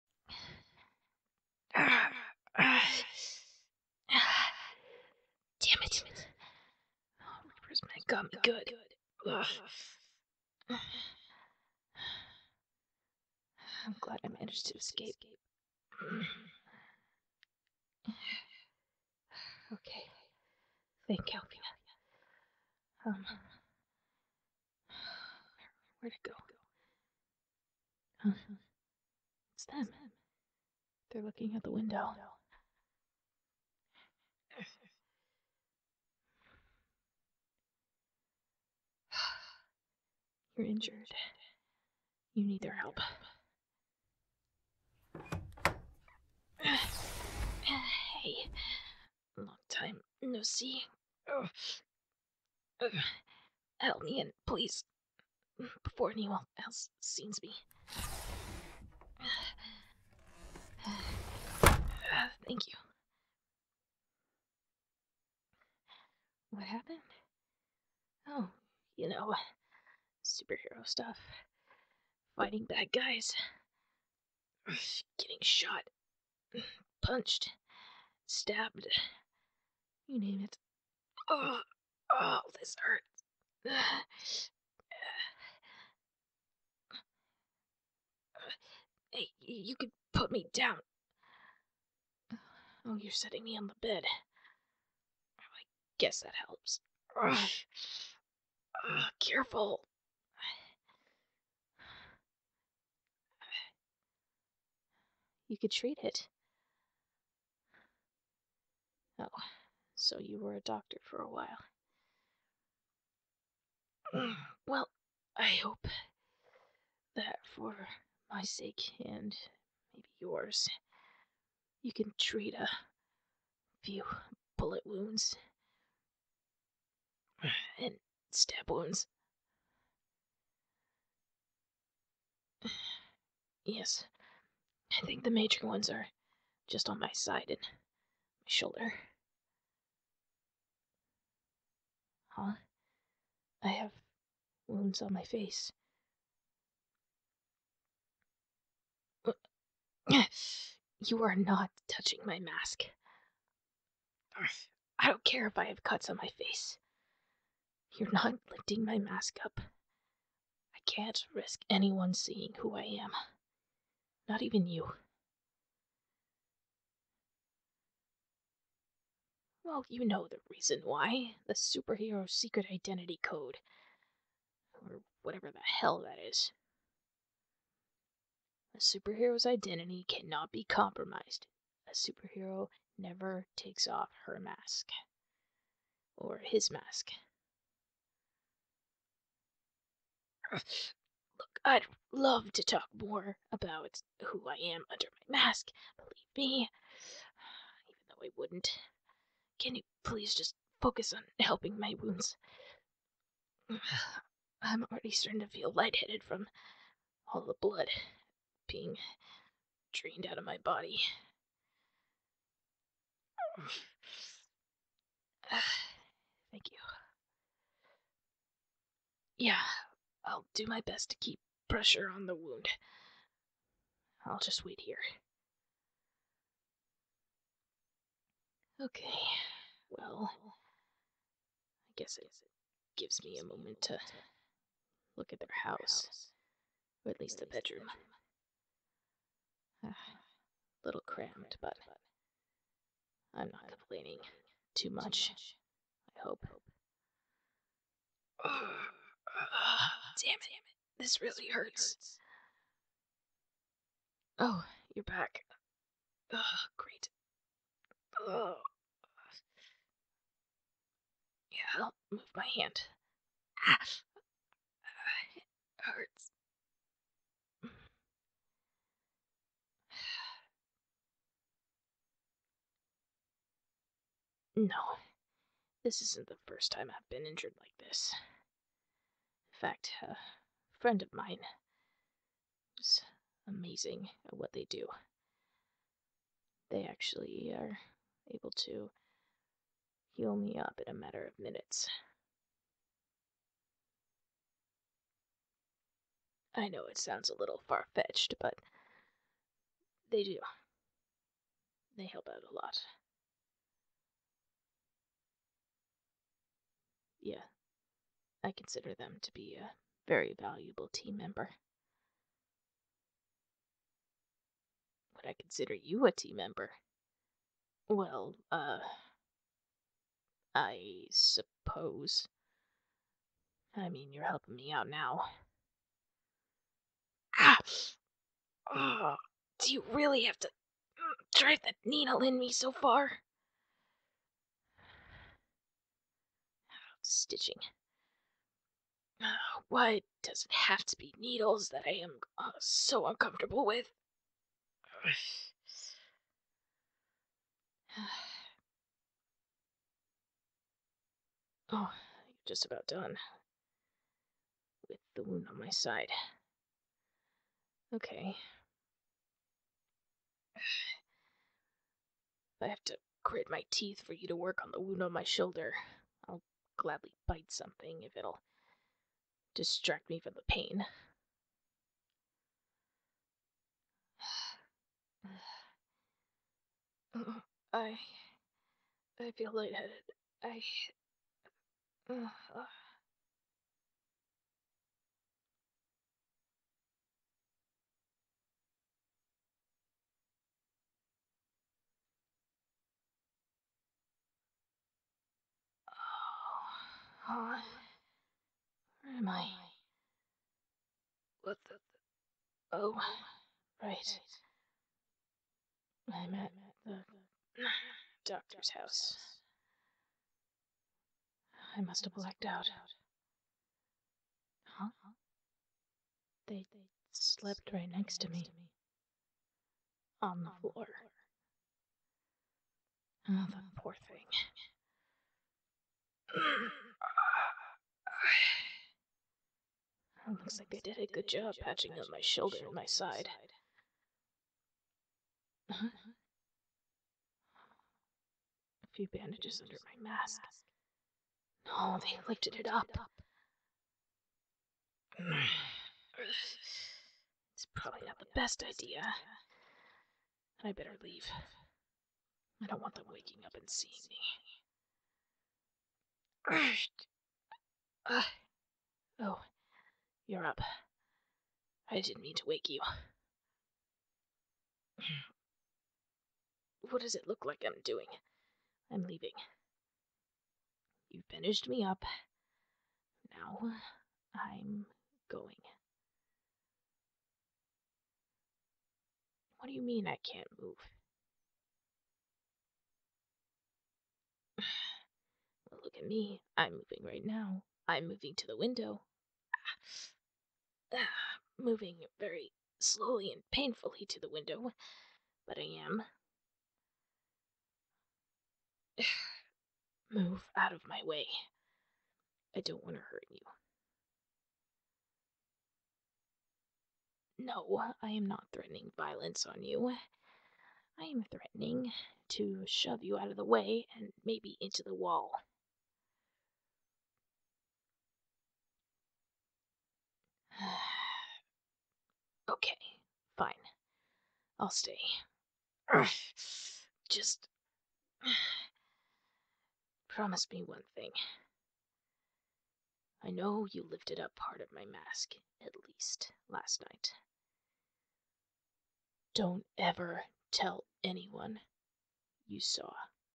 Injured On The Job (ASMR)(Super Hero ASMR)(Tsundere)(Inner Thoughts)
Alphia is injured after a fight with the big bad bad guy who has been fighting her for years, she comes to you for help and hopes that you will not see who she is under her mask